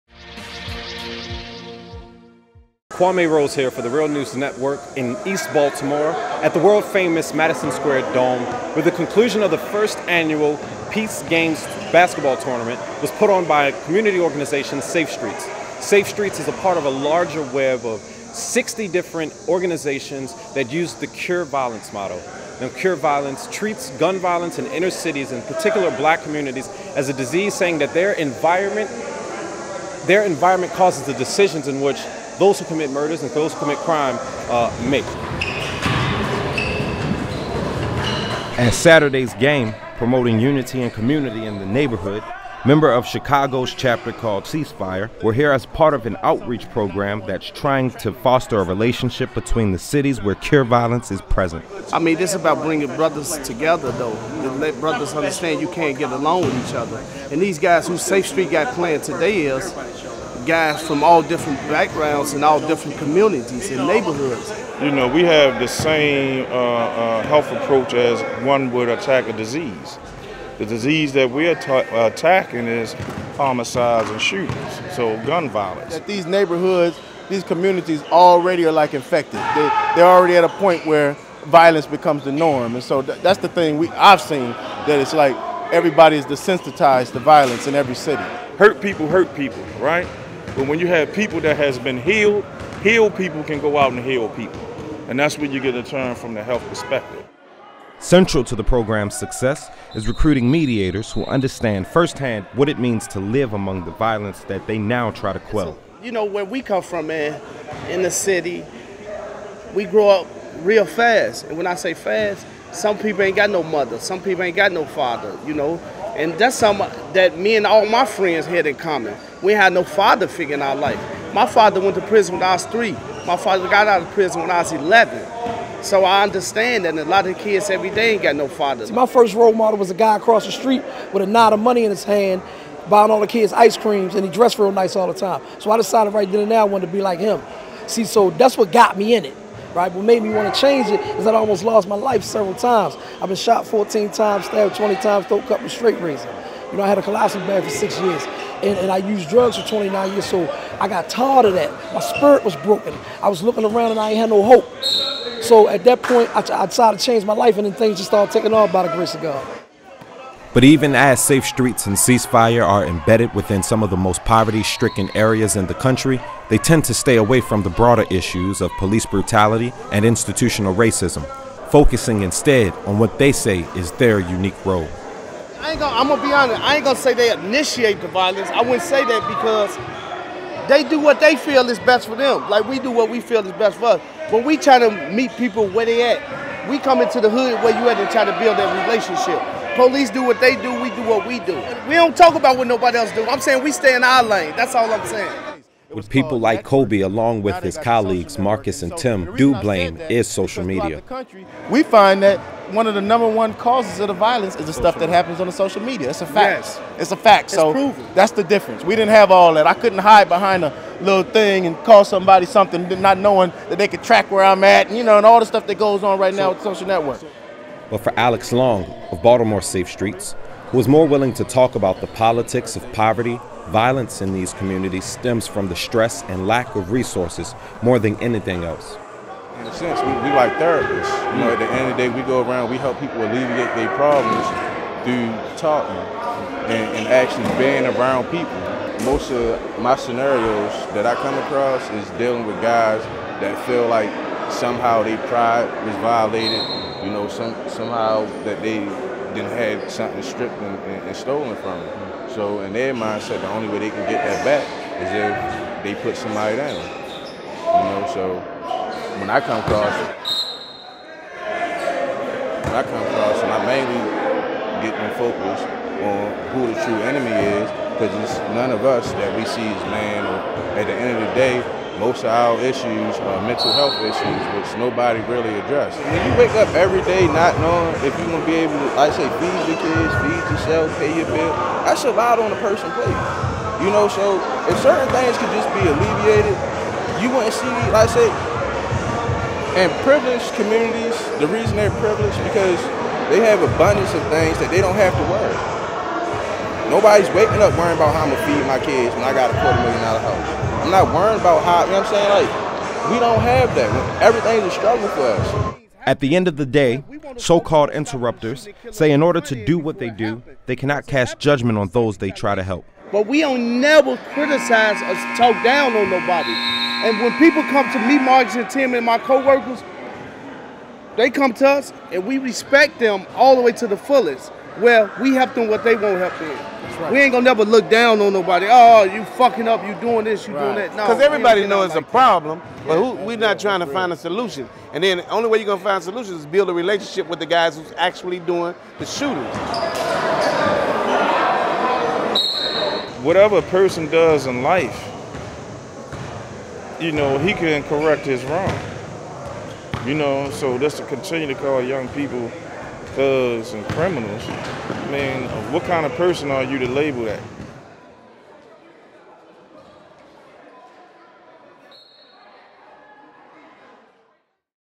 Meet The Ex-Gang Members From Chicago, Baltimore Trying to Keep Blood Off The Streets
Over the weekend former gang members discussed the Cure Violence model started in Chicago, which relies on recruits from poverty-stricken neighborhoods to serve as ‘interrupters’ in the transmission of violence